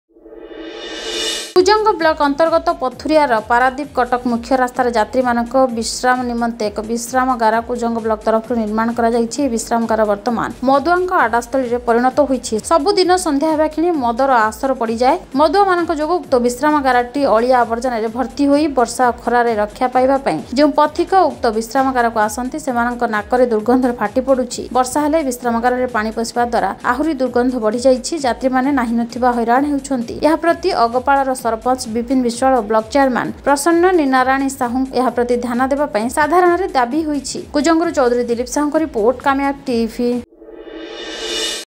ଯାତ୍ରୀ ମାନଙ୍କ ବିଶ୍ରାମ ନିମନ୍ତେ ଏକ ବିଶ୍ରାମଗାର କୁଜଙ୍ଗ ବ୍ଲକ ତରଫରୁ ନିର୍ମାଣ ହୋଇଛି
ଯାତ୍ରୀ ମାନଙ୍କ ବିଶ୍ରାମ ନିମନ୍ତେ ଏକ ବିଶ୍ରାମଗାର କୁଜଙ୍ଗ ବ୍ଲକ ତରଫରୁ ନିର୍ମାଣ ହୋଇଛିkujanga